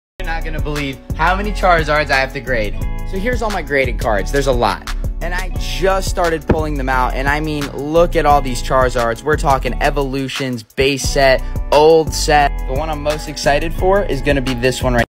I HAVE SO MANY CHARIZARD CARDS! 🔥
►►Today in this video I show you all the Charizard cards I have!

(we cannot send returns if you ask to send things back!)